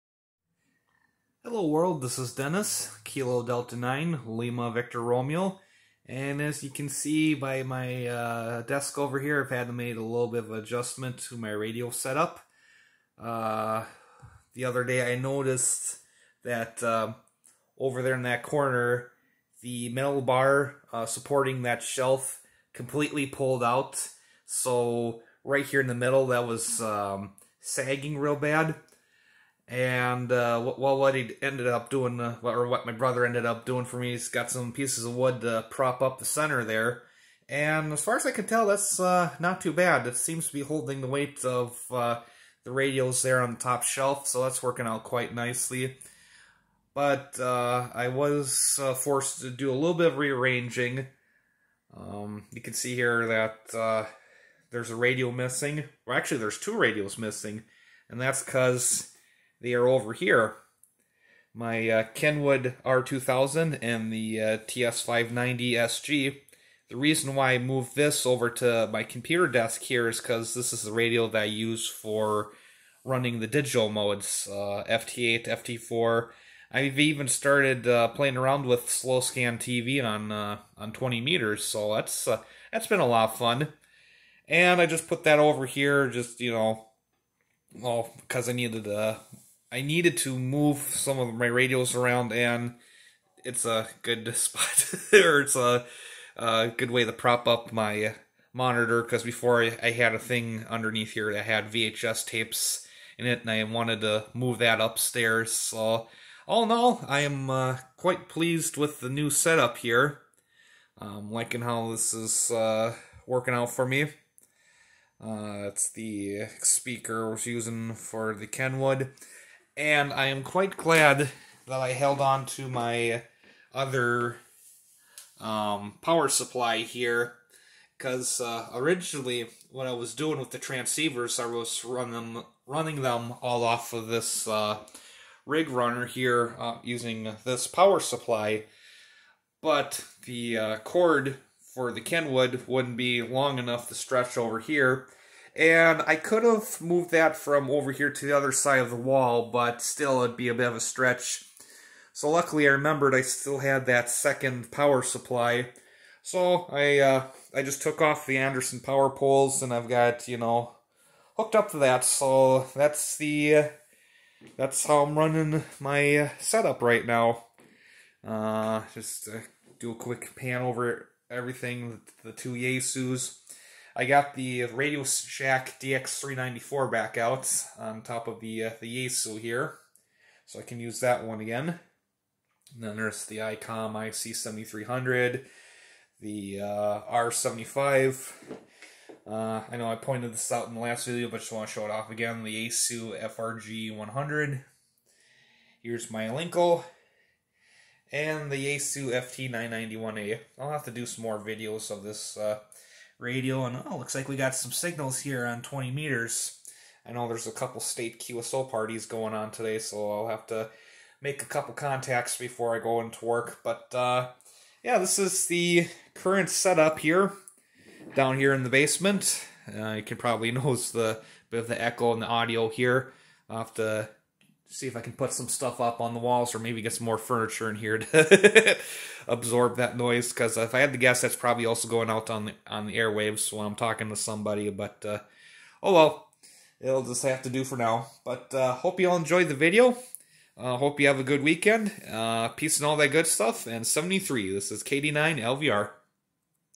KD9LVR'S Radio Shack 10/10/2020
I've had to do some rearranging with my radios here in the shack, but I'm quite happy with the results.